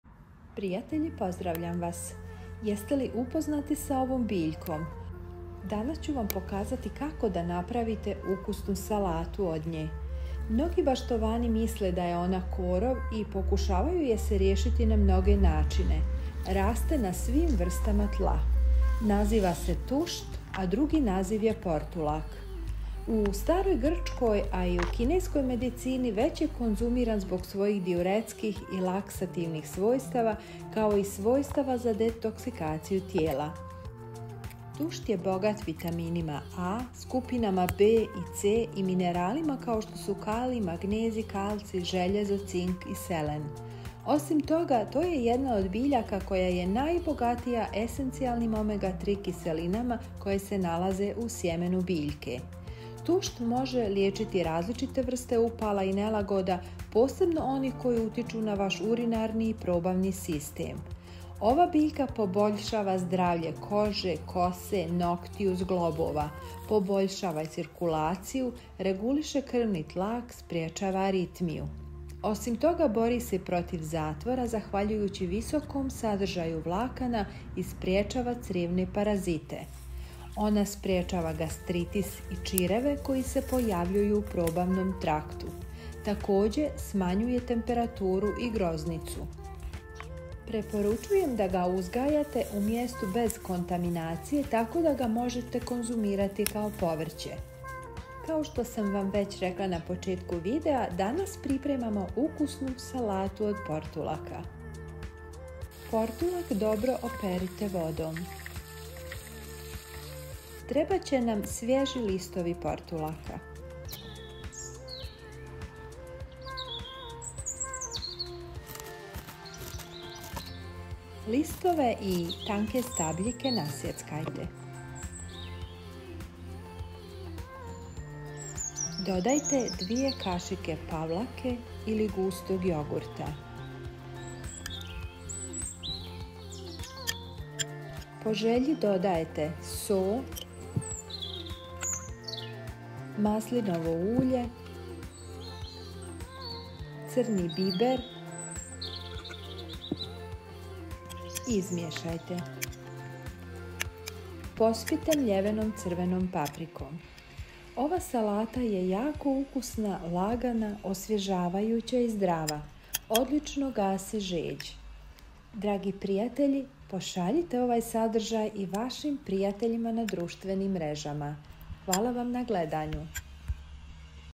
NE PRAVITE GREŠKU I NE ČUPAJTE OVU BILJKU IZ VRTA, nije korov!!!
NE PRAVITE GREŠKU I NE ČUPAJTE OVU BILJKU IZ VRTA, nije korov!!! Velika je vjerojatnost da i u vašem vrtu raste neugledan korov za koji mnogi ne znaju da je, ustvari, jedna od najzdravijih biljaka na svijetu. Riječ je o tuštu, u narodu poznatom i kao tušanj, tušac, tušnjak i portulak, malenoj biljci sa sočnim mesnatim listovima i crvenkastim stabljikama koja cvjeta zvjezdastim žutim cvjetovima. Iako su naše bake vrlo dobro znale kako je riječ o jestivoj i vrlo zdravoj biljci, modernim generacijama malo je poznato da je tušt upravo čudesan izvor proteina, vitamina E i C, a osobito je cijenjen zbog činjenice da sadrži više omega 3 masnih kiselina od bilo koje druge lisnate biljke. Pogledajte de taljnije ovaj video i pošaljite ga i vašim prijateljima na društvenim mrežama. Hvala vam na tome!